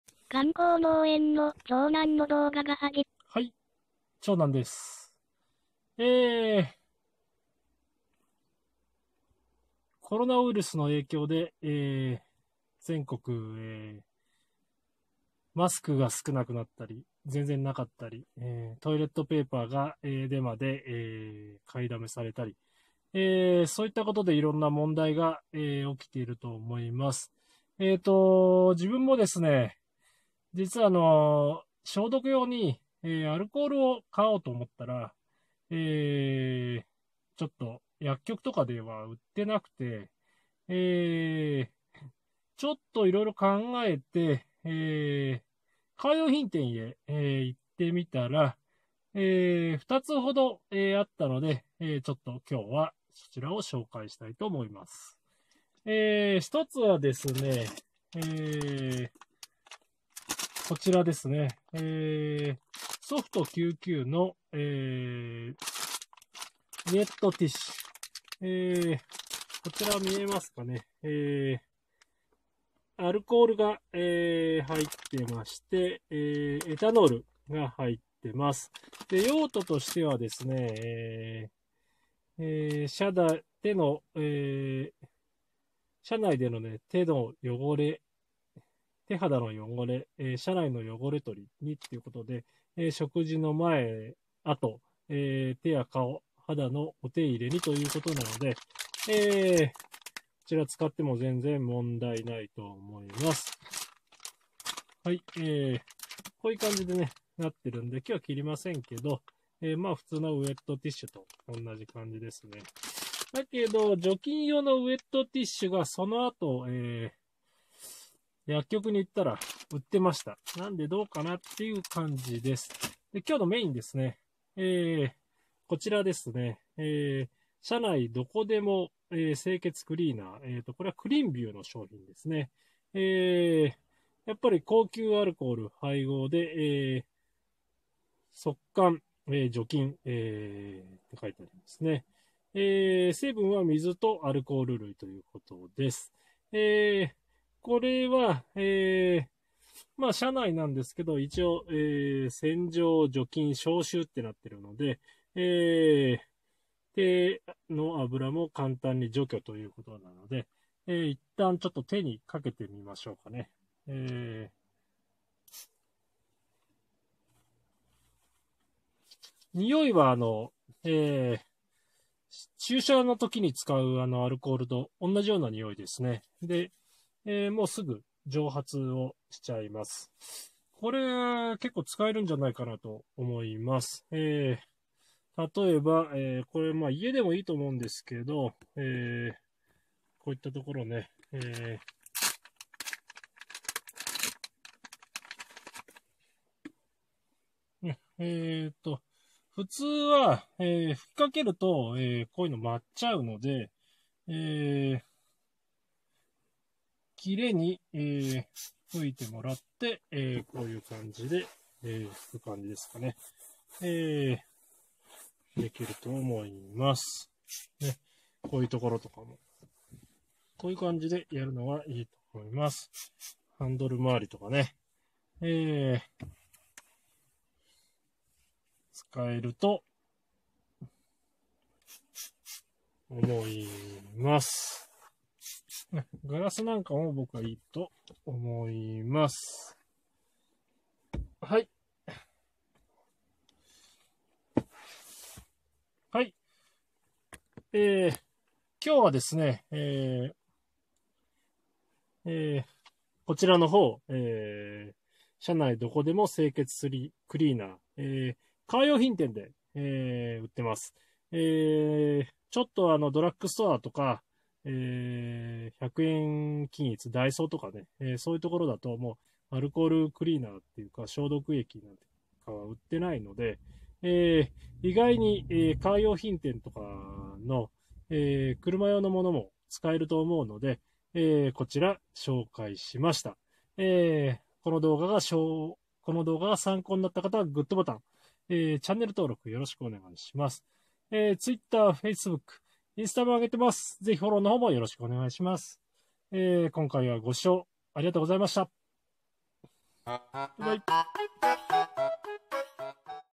カー用品店で買えるアルコール消毒
コロナウイルス騒ぎでアルコール消毒液が買えないのでカー用品店で買って来ました。

観光農園の長男のオススメ動画

動画の中のBGM
FREE BGM DOVA-SYNDROME